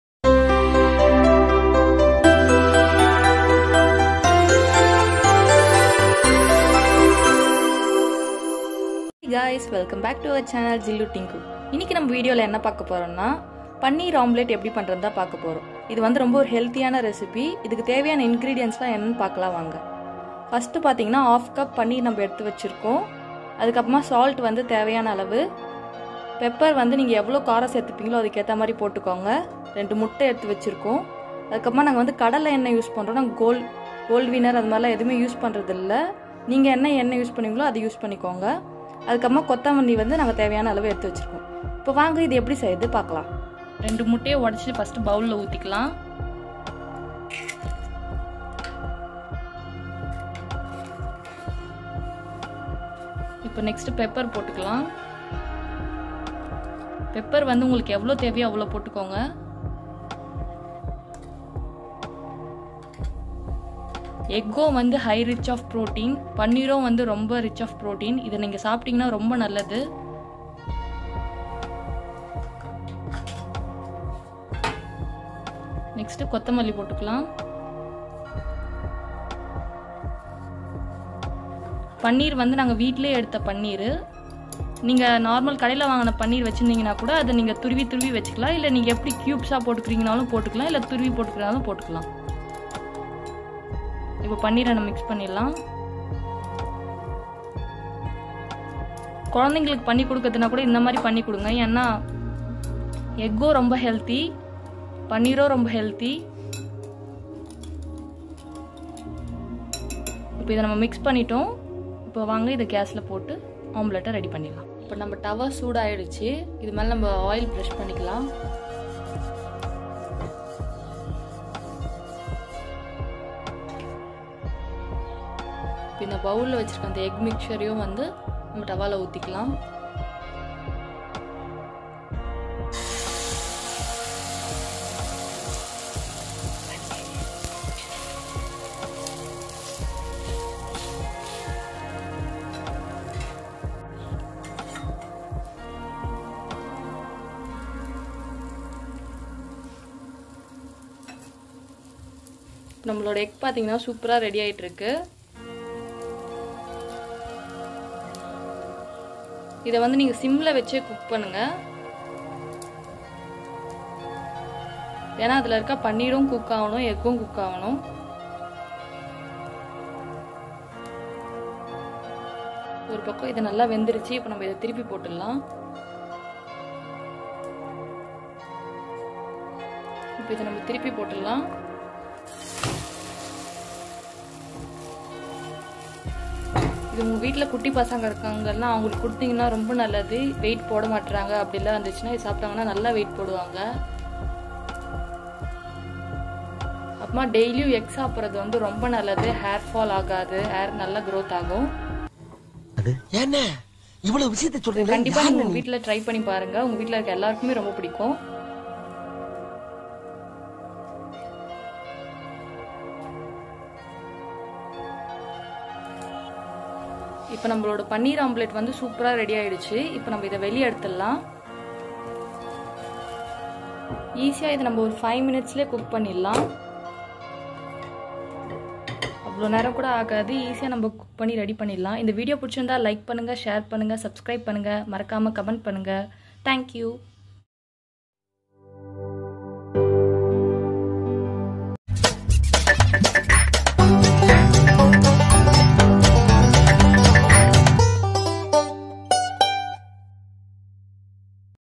Easy 5 minutes break fast | Omelette recipe | Paneer omelette recipe in tamil
Hello! viewers
Here is the recipe for your convenice:-
1) Egg - 2 .
2) Coriander leaves - one hand full or as per your wish.
3) Salt - as per taste.
4) Pepper powder - half teas spoon or as per your  wish.
5) Paneer - half cup .
Hope fully this video is useful for u all.
Today is my sister daughter birthday bless her with your wishes guys.